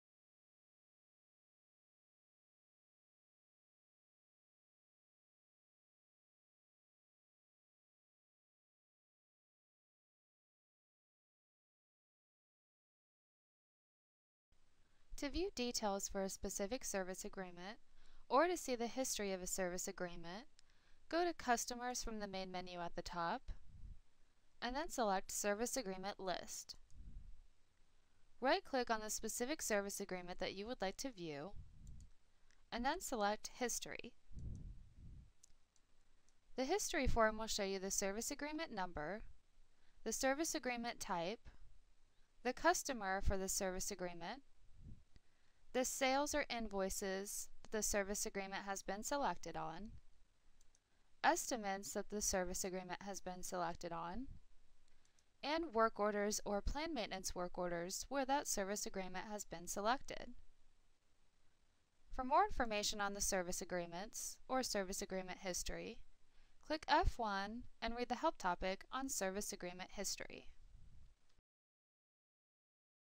How to View Service Agreement History in Total Office Manager®
How to look-up and locate History for a Service Agreement in Total Office Manager.